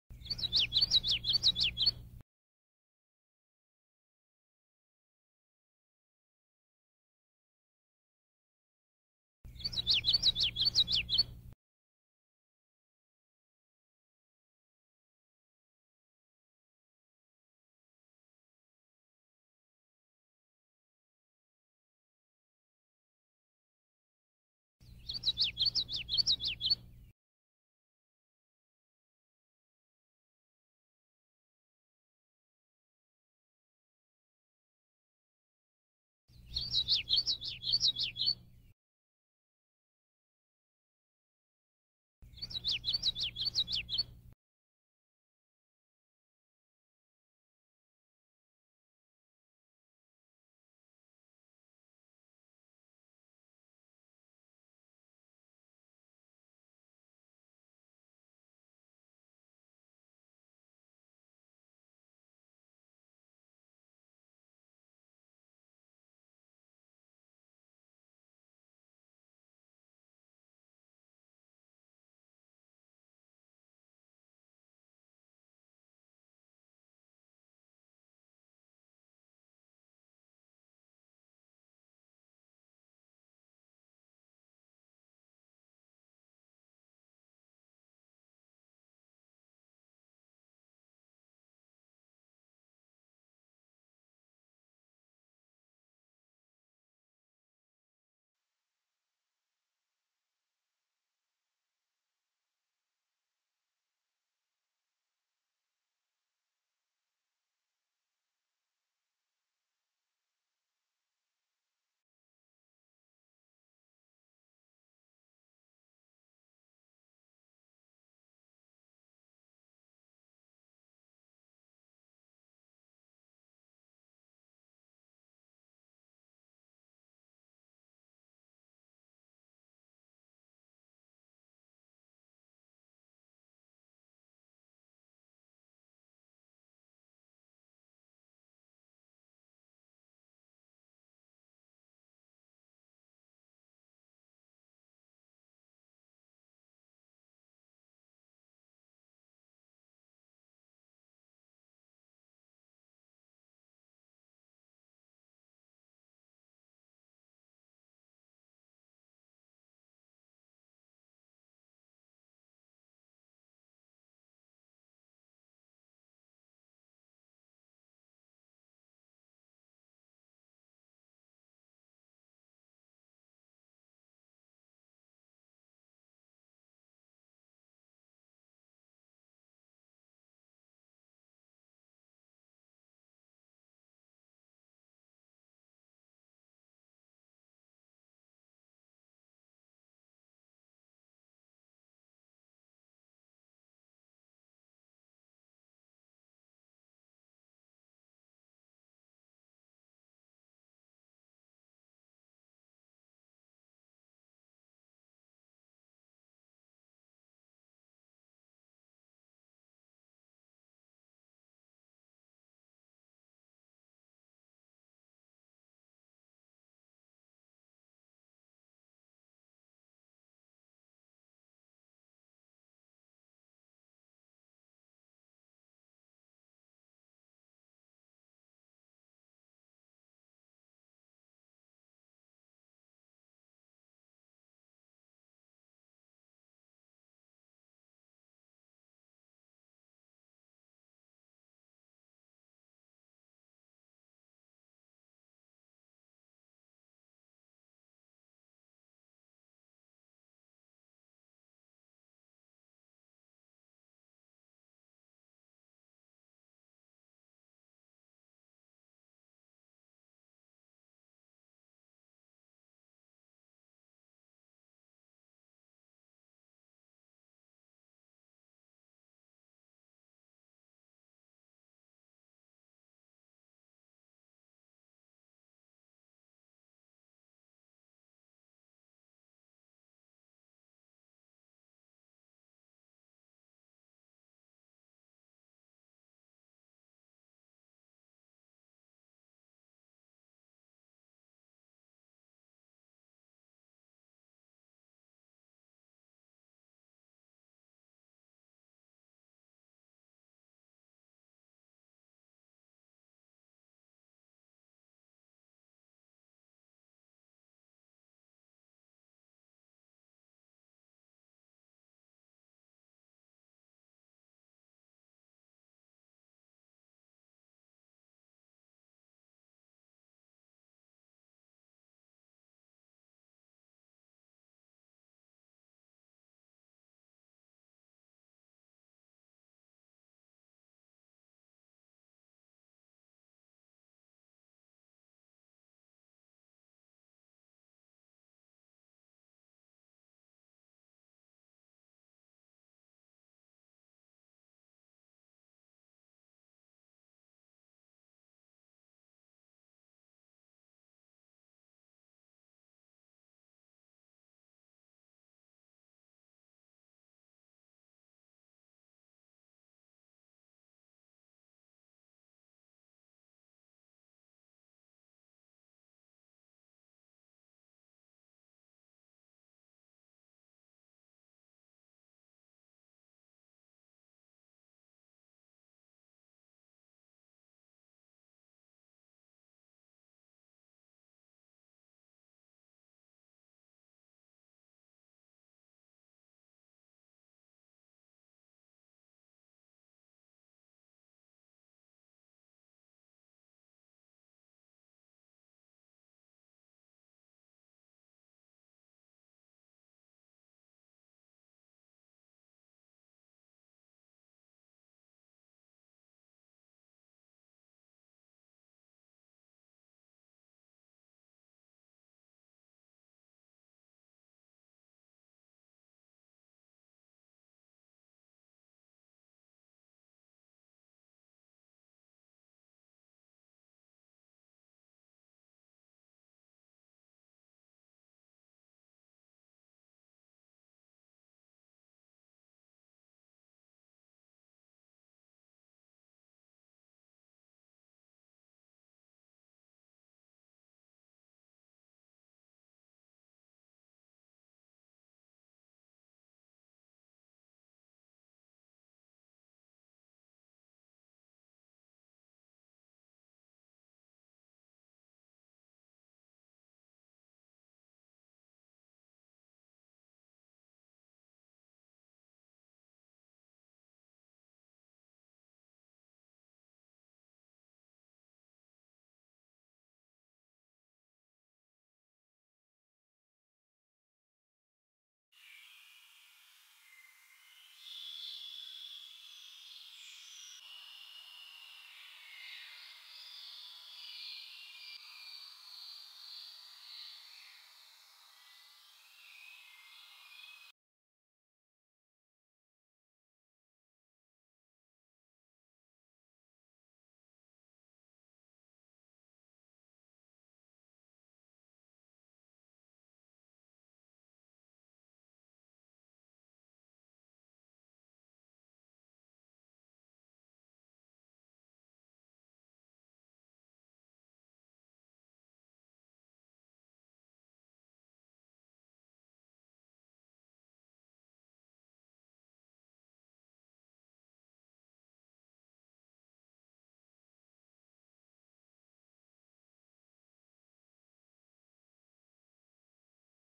COMMON YELLOWTHROAT male singing, foraging Geothylpis trichas
Geothylpis trichas  COMMON YELLOWTHROAT male singing, foraging in Illinois, May, 2019.  Slow motion show what Olympic athletes these are.  Catches insect and just missed one out of reach that escapes.